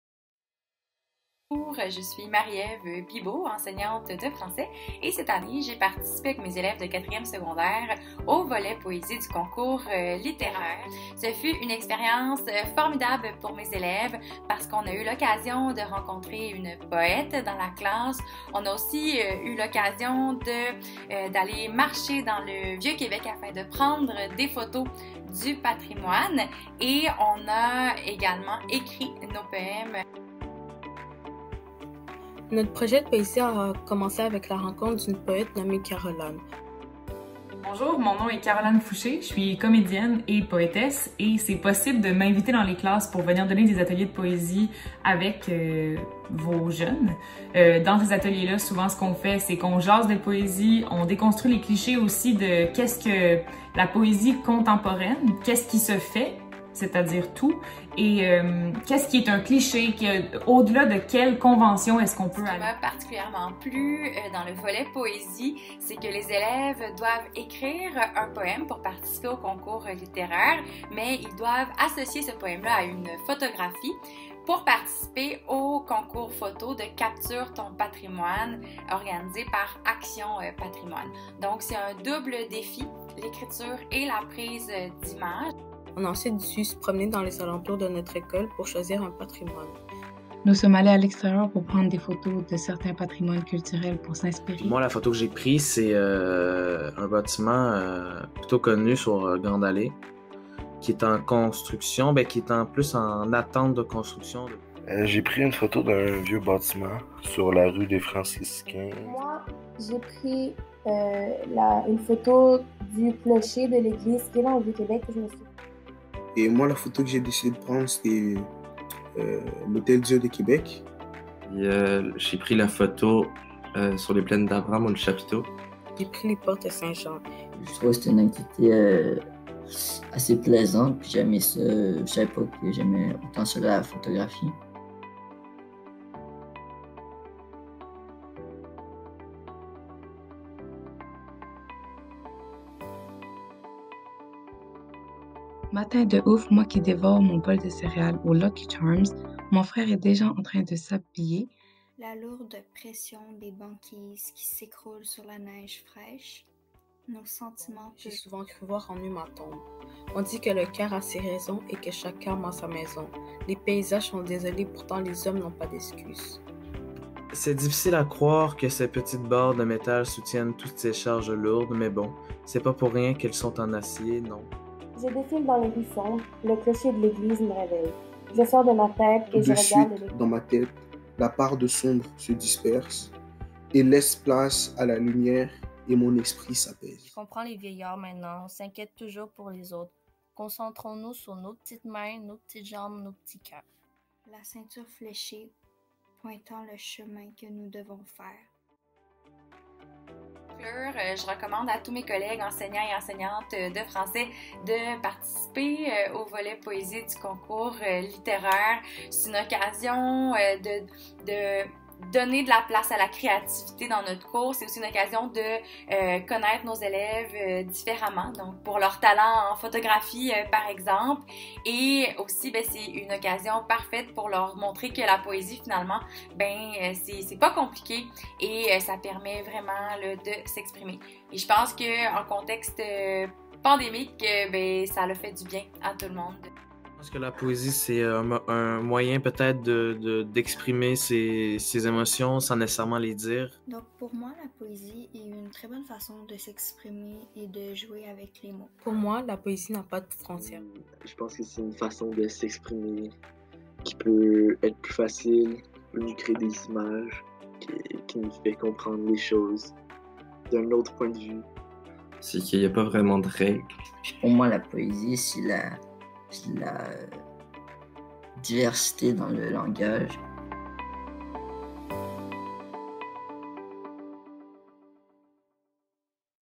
JFP - Présentation du concours littéraire et Capture ton patrimoine
Présentation du volet poésie du concours littéraire du Centre de services scolaire de la Capitale (CSSC) et du concours de photographie Capture ton patrimoine.

Avec la participation des élèves de 4e secondaire de la classe de français de Madame Marie-Ève Bibeau. École secondaire Joseph François Perrault.

Hiver 2021.